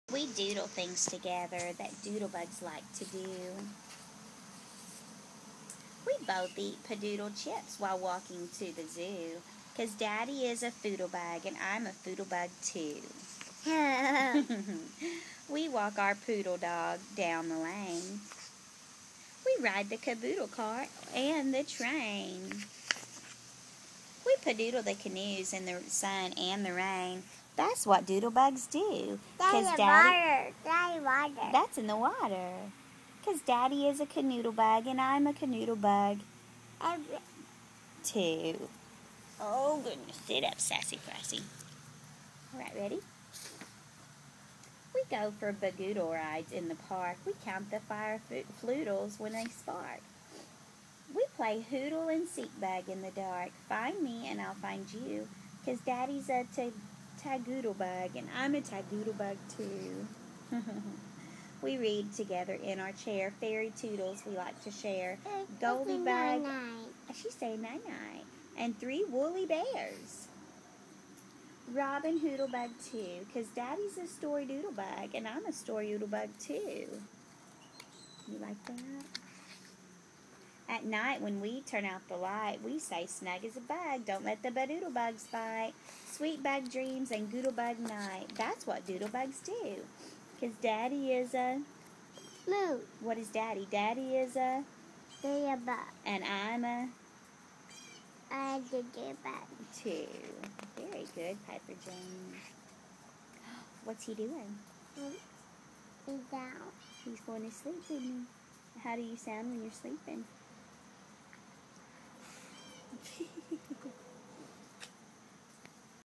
Story time with Piper Jane
Reading daddy is a doodlebug with Piper.